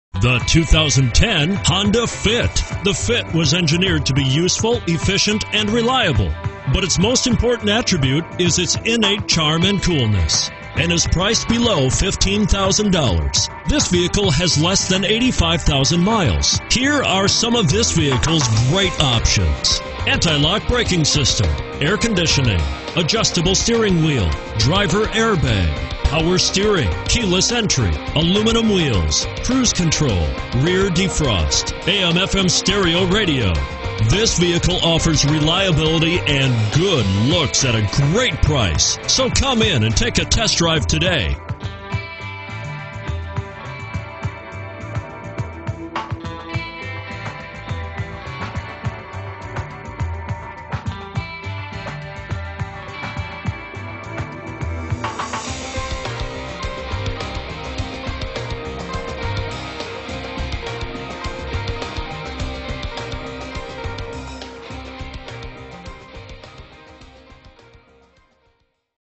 2010 Honda Fit Colmar PA 14M2112B
2010 Honda Fit Sport - $11450

Looking for the right car? Today could be your lucky day. With 81091 miles, this Storm Silver Metallic, 2010 Honda Fit equipped with Automatic transmission could be yours. Request more information and set up a test drive right away.